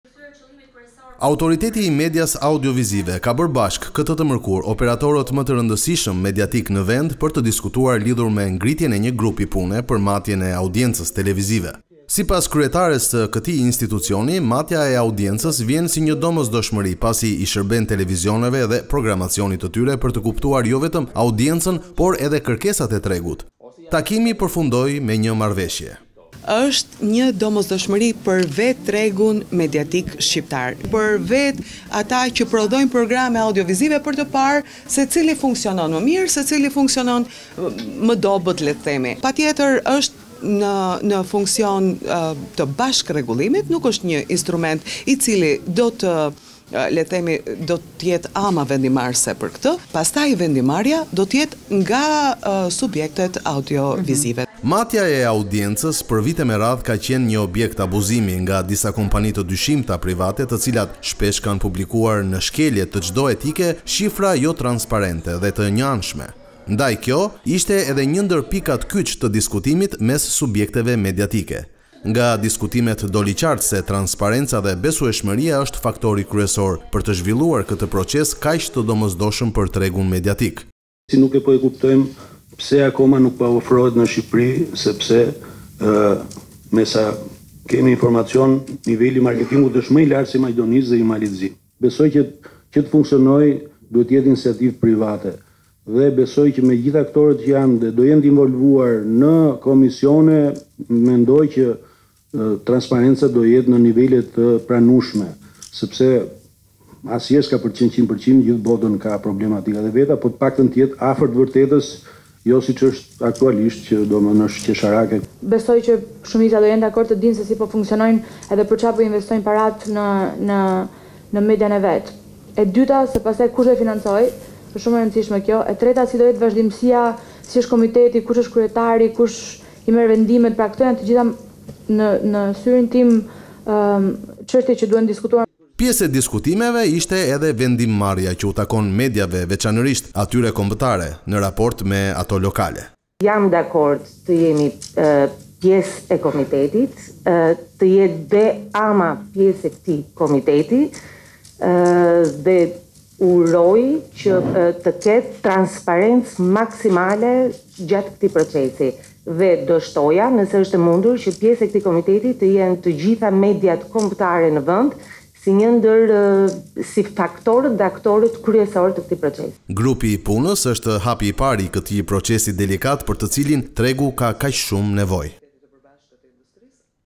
Matja e audiencës televizive, AMA dhe operatorët privatë bien dakord për ngritjen e një grupi pune
Autoriteti i Medias Audiovizive ka bërë bashkë këtë të mërkurë operatorët më të rëndësishëm mediatik në vend për të diskutuar lidhur me ngritjen e një grupi pune për matjen e audiencës televizive. Sipas kryetares së këtij institucioni matja e audiencës vjen si një domosdoshmëri pasi i shërben televizioneve dhe programacionit të tyre për të kuptuar jo vetëm audiencën por dhe kërkesat e tregut. Takimi përfundoi me një marrëveshje.

“Ngritja e një grupi pune është një domosdoshmëri jo për institucionin rregullator por është domosdoshmëri për vetë tregun mediatik, për të parë se cili funksionon më mirë dhe cili funksionon më dobët. Është një instrument që shumë vende jo vetëm në Europë por dhe në rajon e ka implementuar. është në funksion të bashkërregullimit dhe ne do të jemi vetëm në rolin e koordinimit ndërsa vetë institucionet e mendrave do të kenë fuqinë vendimmarrëse”, u shpreh kryetarja e AMA-s Armela Krasniqi.

Matja e audiencës për vite më radhë ka qenë një objekt abuzimi nga disa kompani të dyshimta private, të cilat shpesh kanë publikuar në shkelje të çdo etike shifra jotransparente dhe të njëanshme. Ndaj kjo ishte dhe një ndër pikat kyç të diskutimit mes subjekteve mediatike. Nga diskutimet doli qartë se transparenca dhe besueshmëria është faktori kryesor për të zhvilluar këtë  proces kaq të domosdoshëm për tregun mediatik.

"Dihet që niveli i marketingut në Shqipëri është me i lartë se ai i Maqedonisë. Por çështja është se ku do i gjejmë ne fondet. Pjesa tjetër,  organizimi, transparenca vijnë më pas. Gjeja më e thjesht është shohim sa kushton projekti si do ndahet pastaj bërja e strukturave, a do ketë përfaqësues nga qeveria. Por qe te funksionoj duhet të jetë private. Transparenca do jetë e pranueshme afër te vërtetës dhe jo siç është, qesharake”, u shpreh CEO i Vizion Plus Genc Dulaku.

Pjesë e diskutimeve ishte dhe vendimmarrja që u takon mediave veçanërisht atyre kombëtare, në raport me ato lokale.

Grupi i punës është hapi i parë i këtij procesi delikat për të cilin tregu ka kaq shumë nevojë./vizionplus.tv